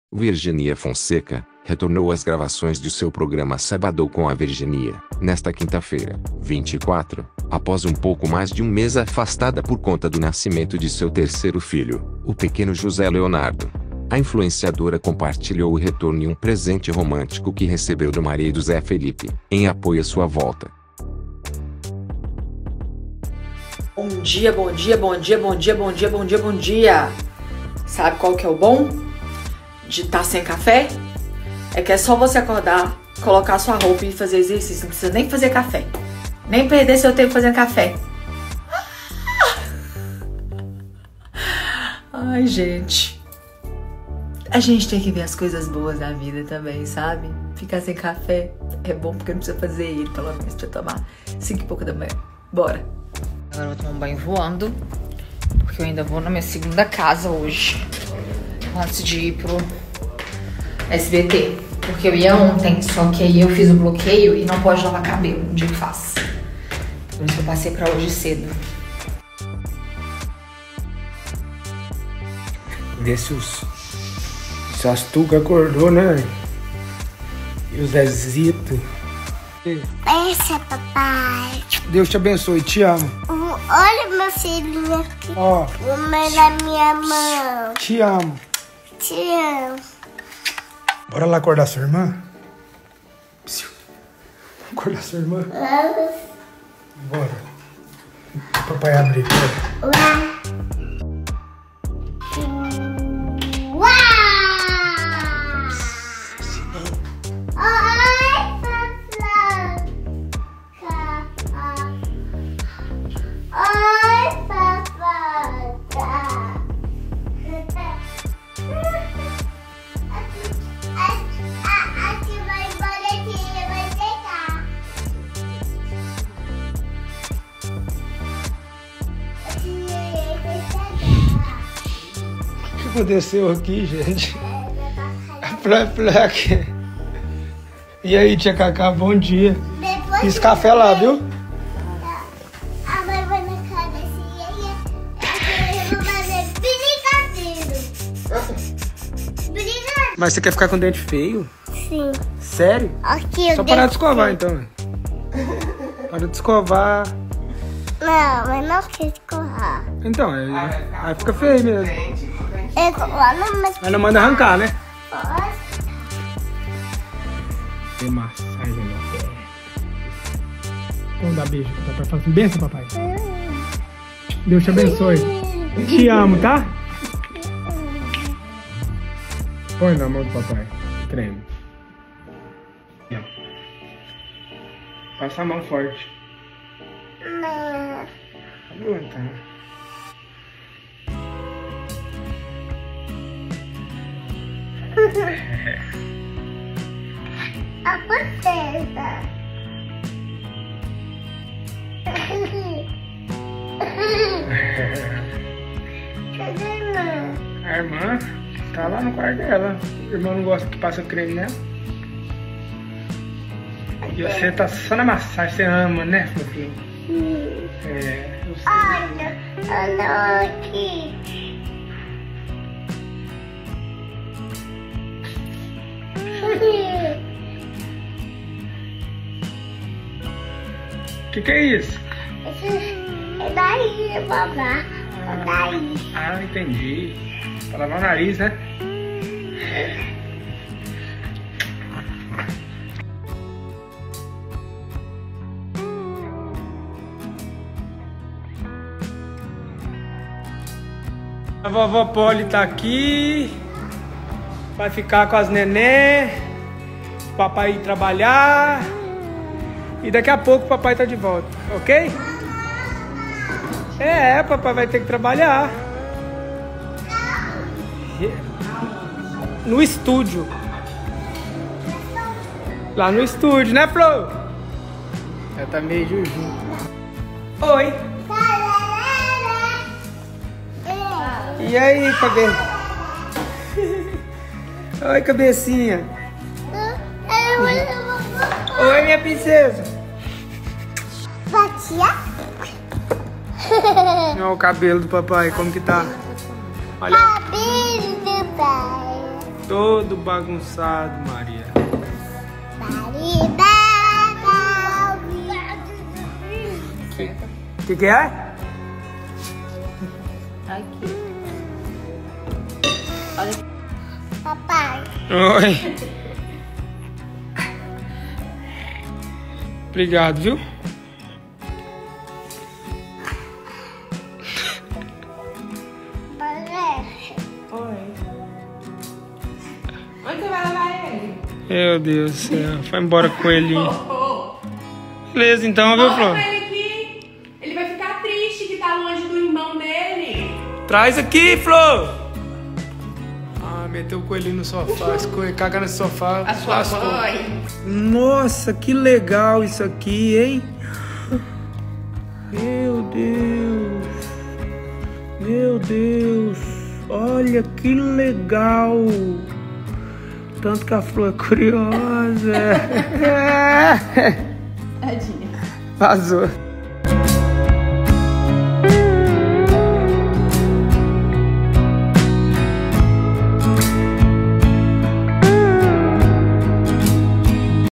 Virginia Fonseca volta a gravar seu programa e recebe presente romântico de Zé Felipe
Virginia Fonseca
Presente romântico
Zé Felipe
Programa